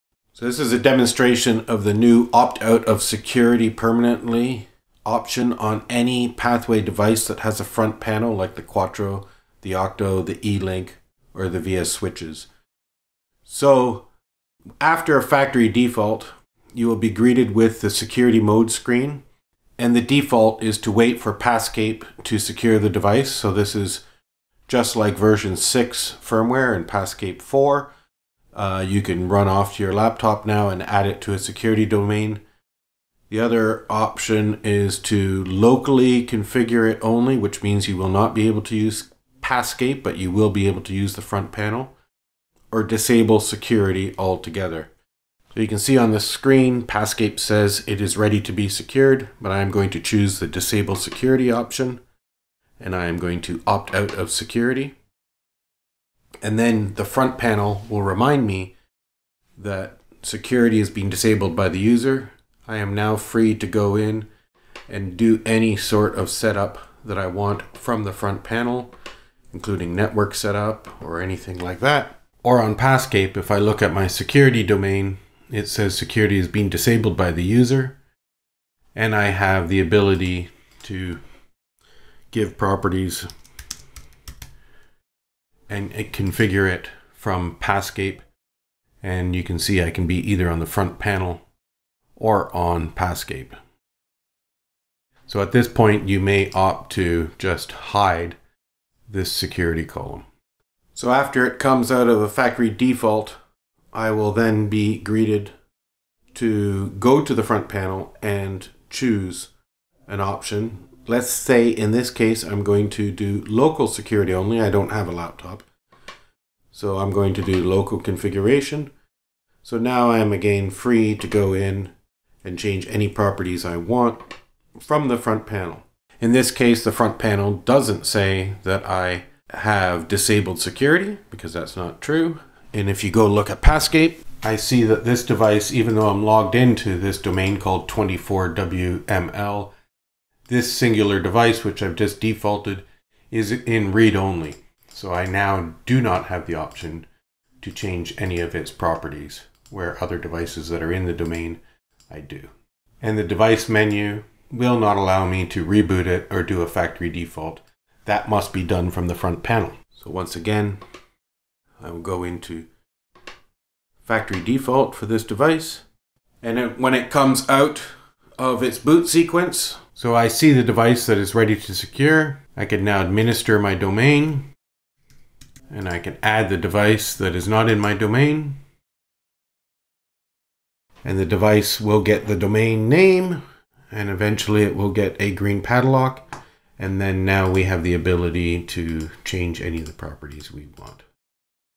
Opting Out of Security
For Pathway Connectivity devices with a front UI (QUATTRO, OCTO and VIA rack mount gear), firmware v6.1 allows an end user to permanently opt out of the security features offered by Pathscape. This will allow any unauthorized user on the network the ability to make configuration changes to devices. It should only be used on dark networks and with knowledge of the risks involved. Note, eLink's does not have a v6.1 release. Please watch this space.